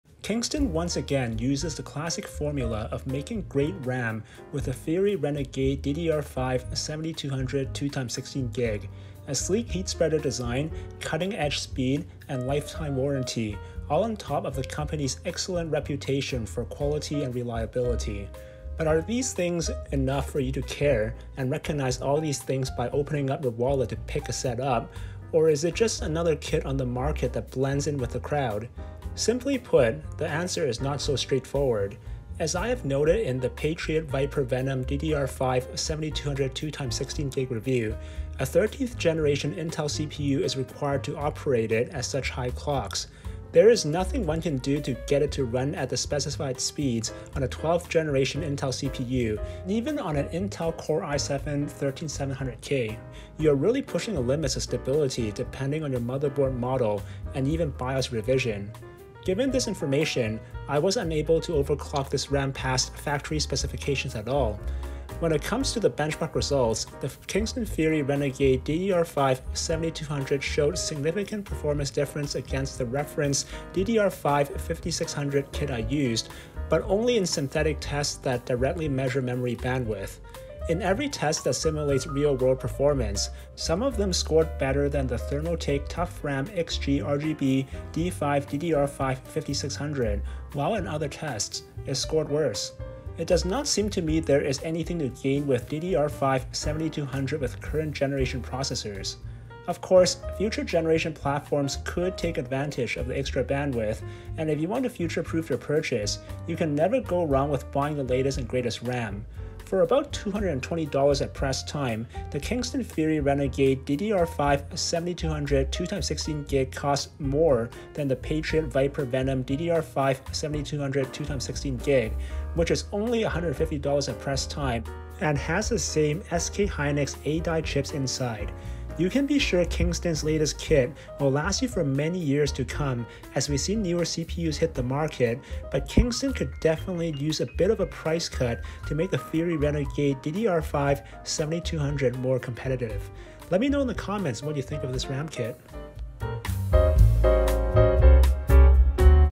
DDR5-7200 on i7-13700K tested - Kingston FURY Renegade DDR5-7200 2x16GB Review
The Kingston FURY Renegade DDR5-7200 2x16GB RAM offers the latest in DDR5 memory speed with cool looking heatspreaders on the outside and SK hynix A-die chips on the inside.